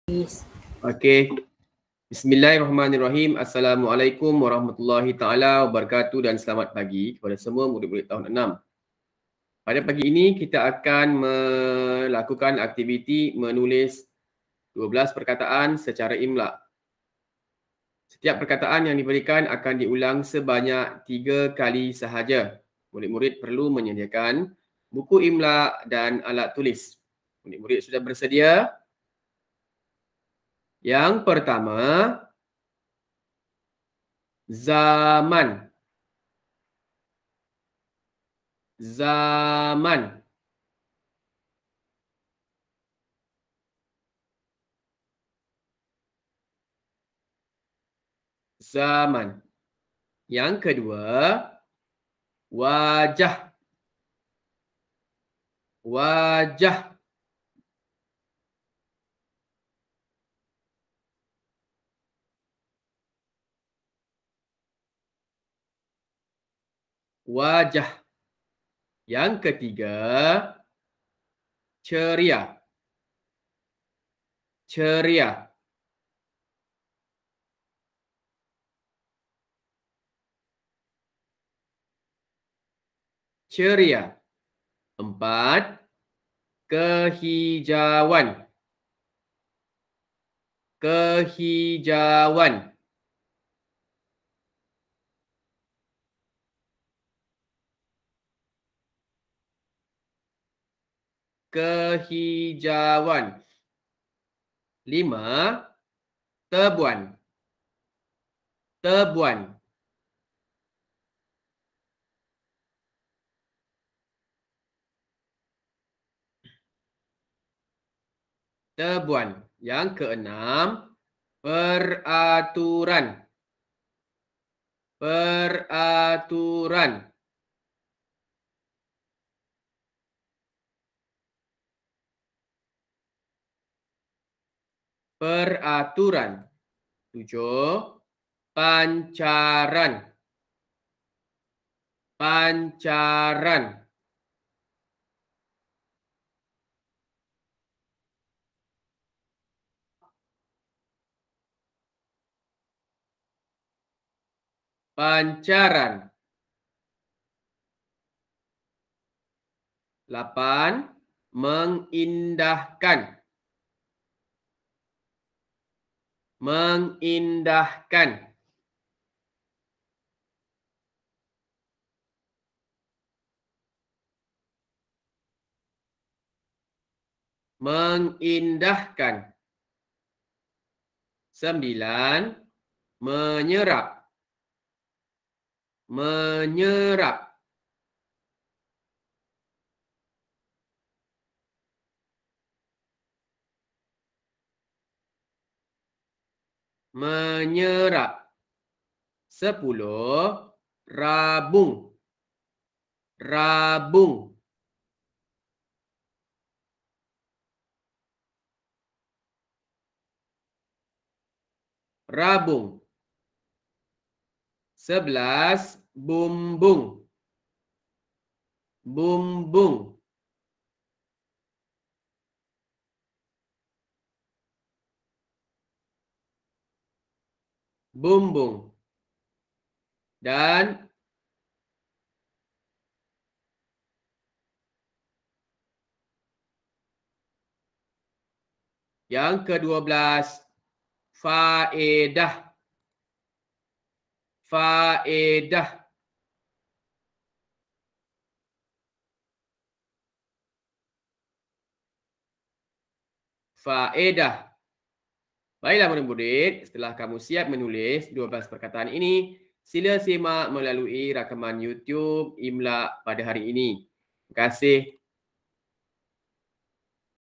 Imlak 25 BM Tahun 6 SJK (27 Oktober 2021)
Menulis 12 perkataan secara imlak.